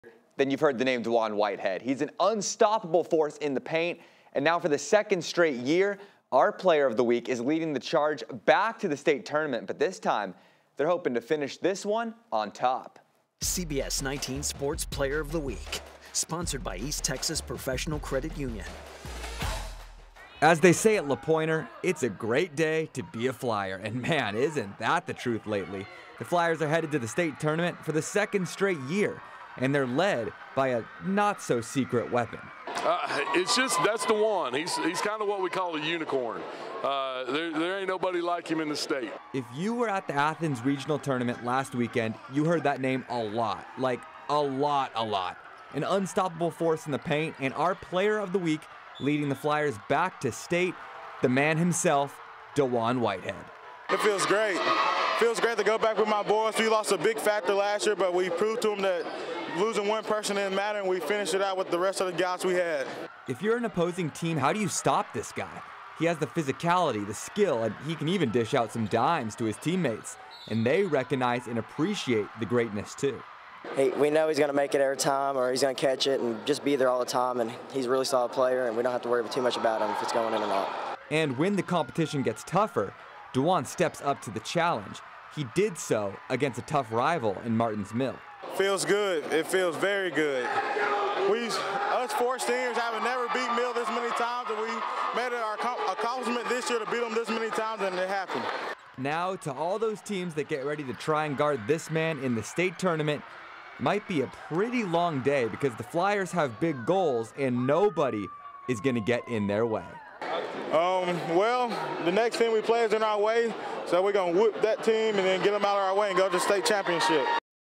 East Texas Professional Credit Union Player of the Week: LaPoynor's Dijuan Whitehead
The senior big man is leading the Flyers back to the state tournament for the second straight year.